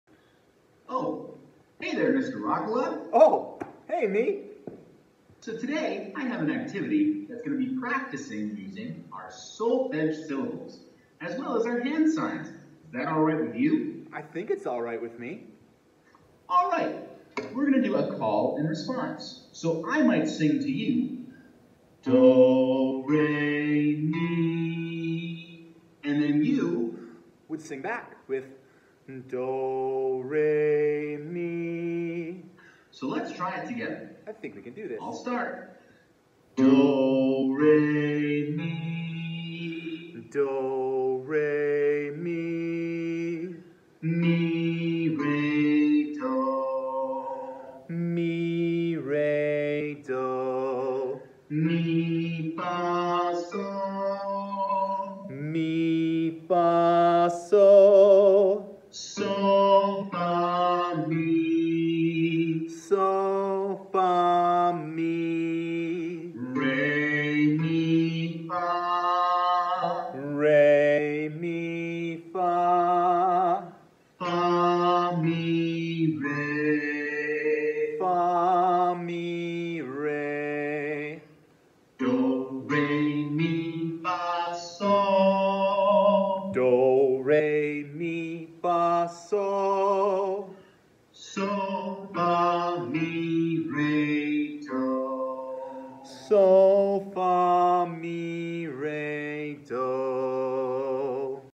Practice with do, re, mi, fa, sol : call and response.
Feel to join in at home to practice your solfege and hand signs!